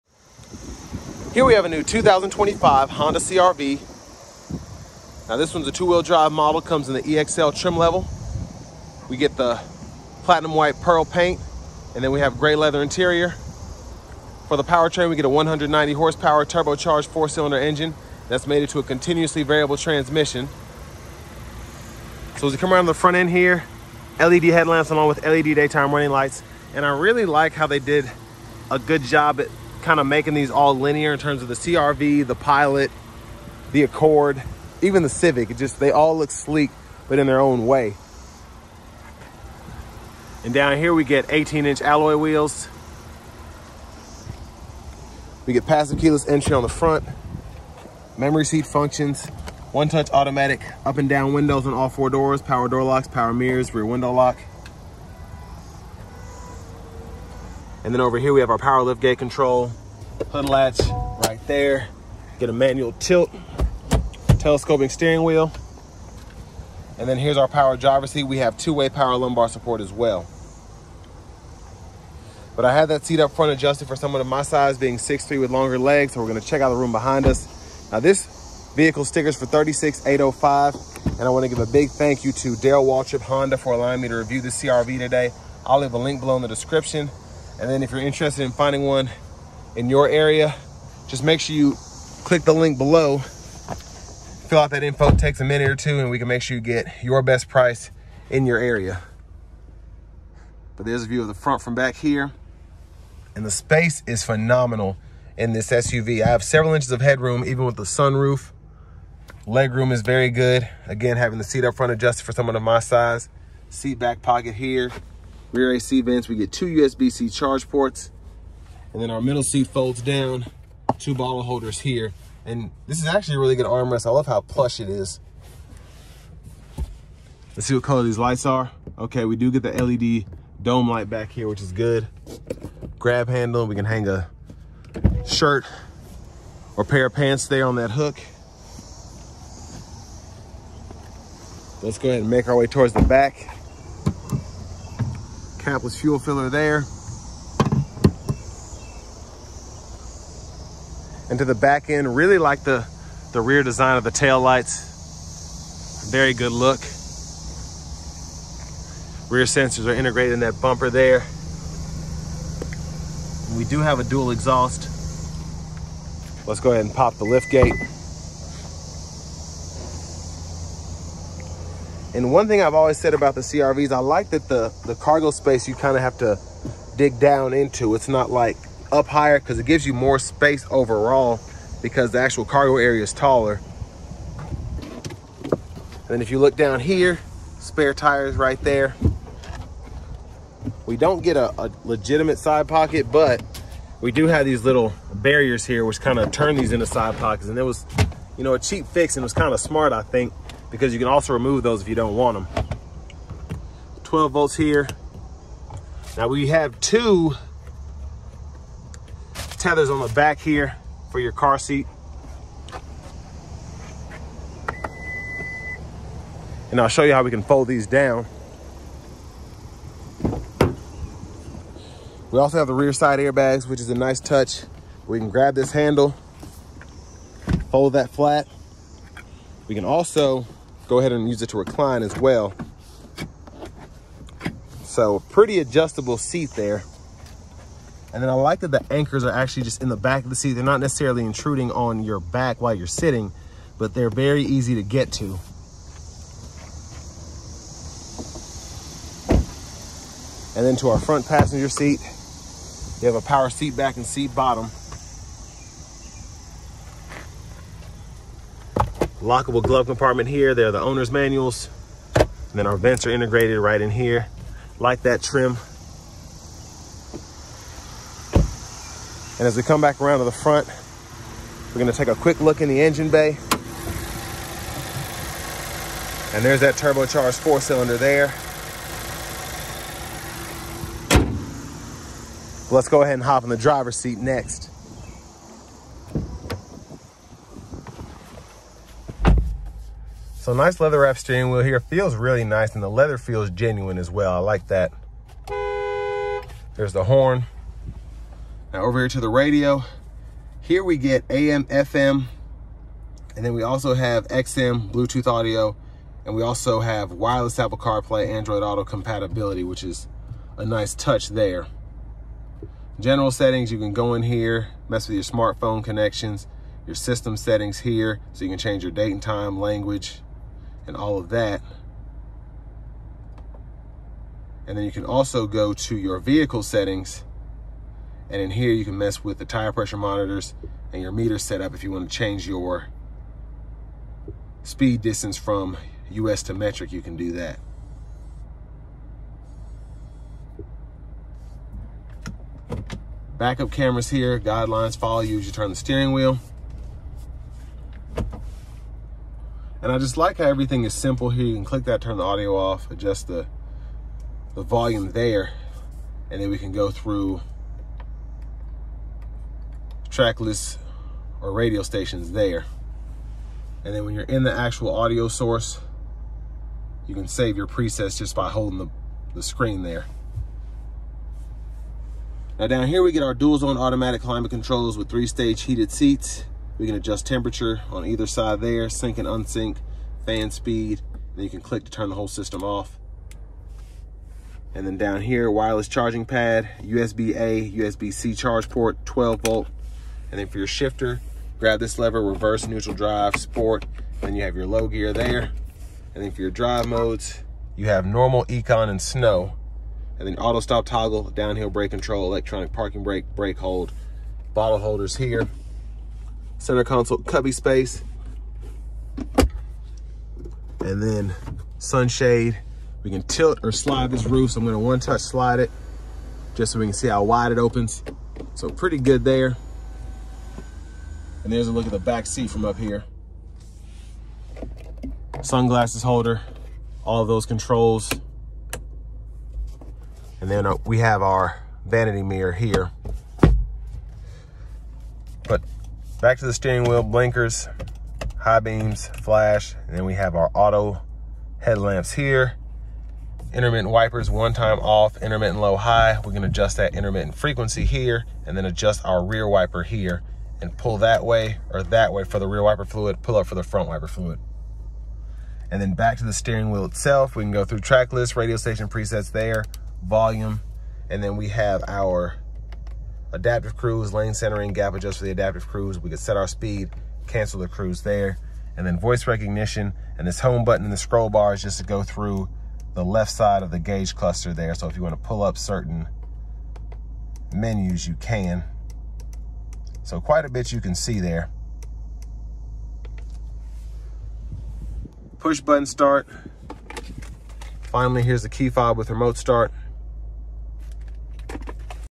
2025 Honda CR-V EX-L Walkaround (Features, Interior, Exterior)
LOOKING TO FIND THE BEST DEAL ON A NEW VEHICLE IN YOUR AREA?  MAKE YOUR LOCAL DEALERS COMPETE ON PRICE AND AVAILABILITY BY FILLING OUT YOUR INFORMATION HERE: